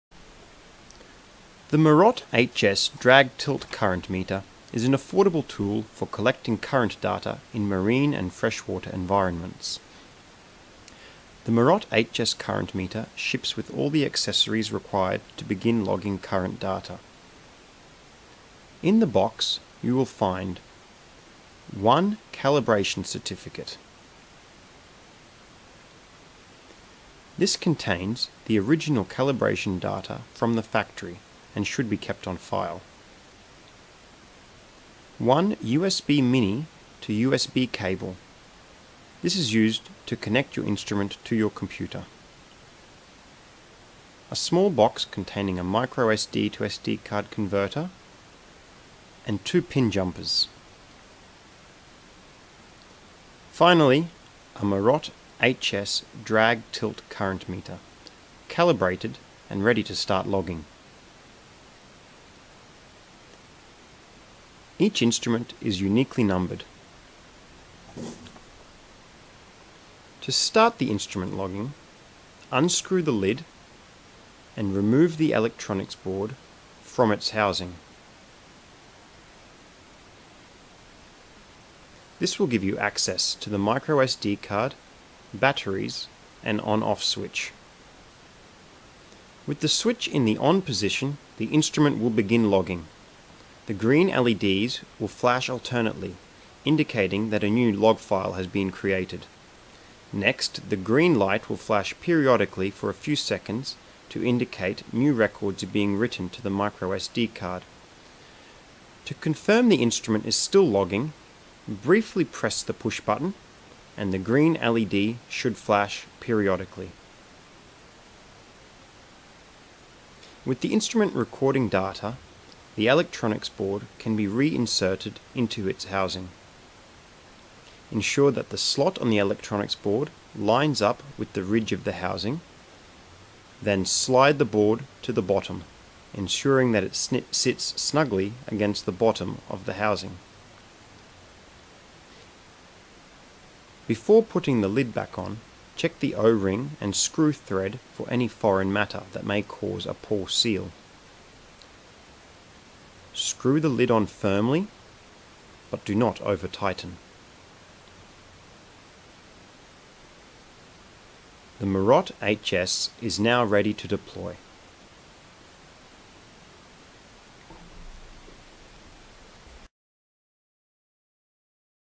Getting Started with the Marotte HS Current Meter
This video outlines how to get started with the Marotte HS Current Meter to supplement the manual available for download at: